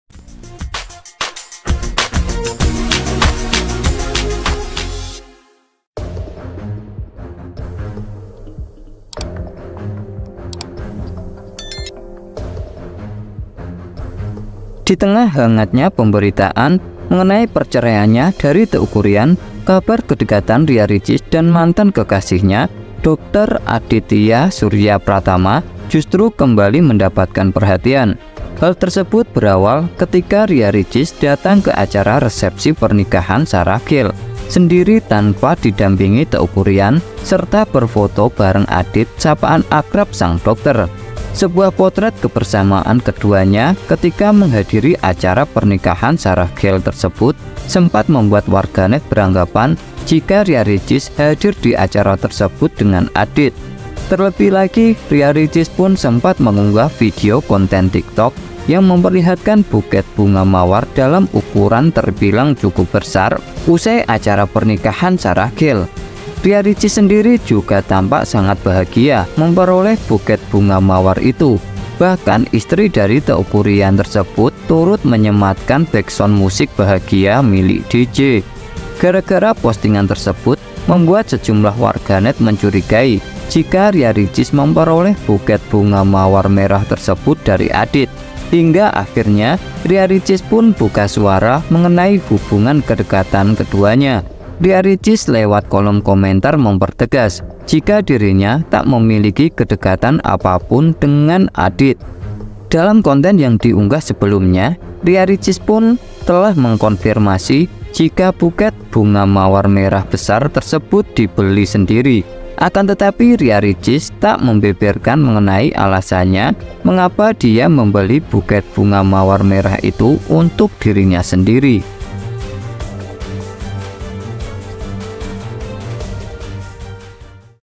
Klarifikasi Ria Ricis soal Kedekatannya Dgn Dokter Aditya Surya Pratama Usai Gugat Cerai Teuku Ryan
Klarifikasi Ria Ricis Soal Kedekatannya Dengan Dokter Aditya Surya Pratama Usai Gugat Cerai Teuku Ryan